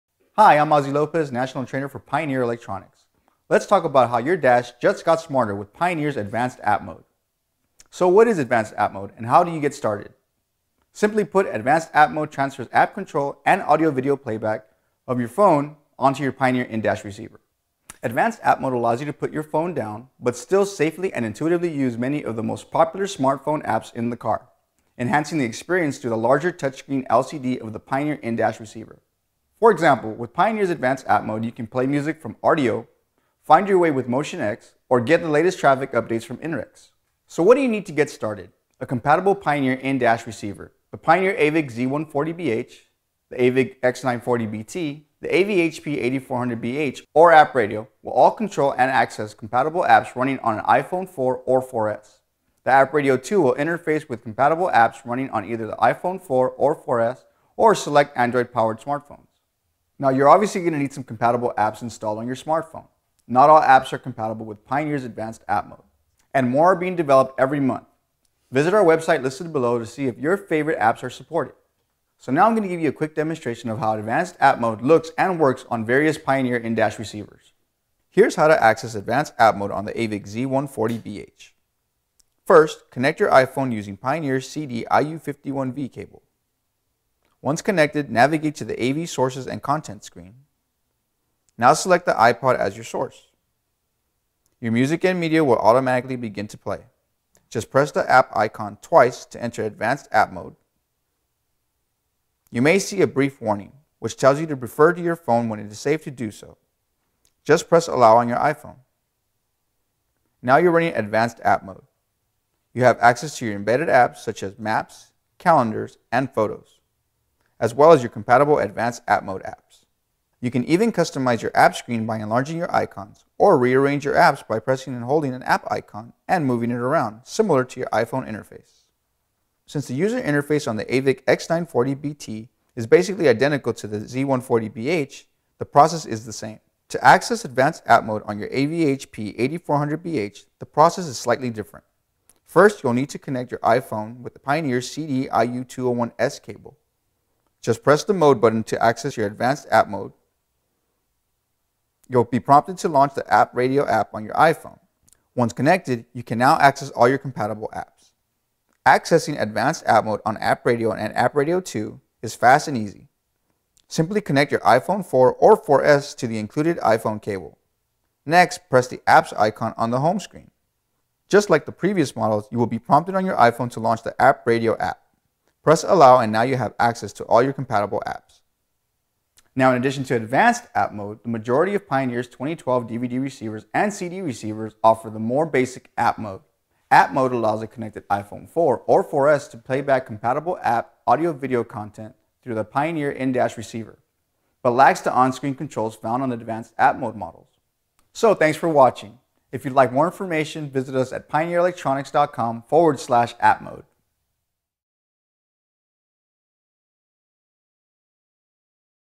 Advanced App Mode Tutorial
Learn how you can view and control app based content from your smartphone right on the screen of compatible 2012 Pioneer in-dash receivers.